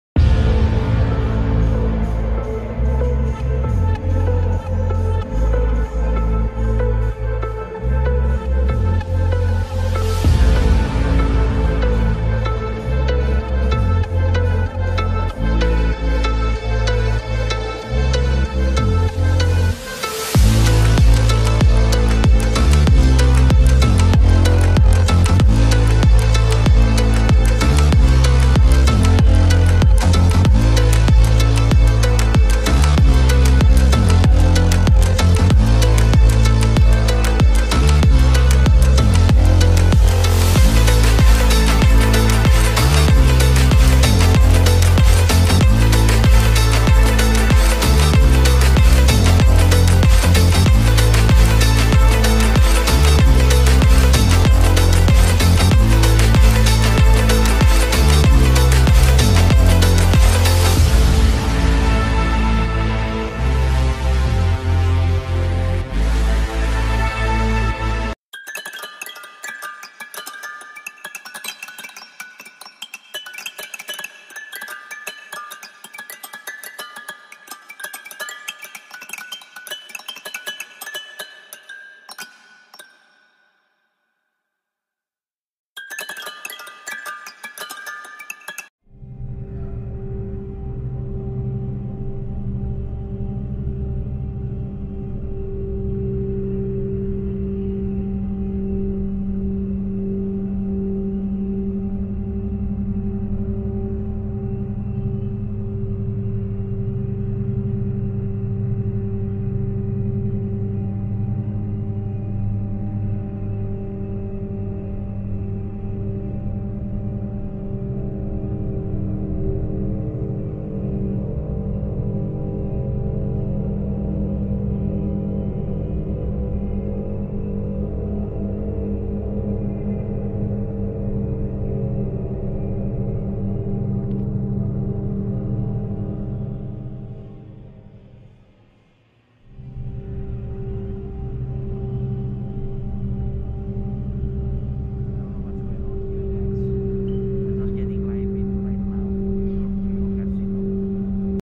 Hell unleash the invisible demons
No 10 of the Mysteries Of City's buildings or statues stories and myths blended together on that story of :The mystery of the Hellbound breathe demonic myst spreading in few Night's of the floors of New York New York Casino hotel in Las Vegas Nevada coming from underground i believe caves never get explore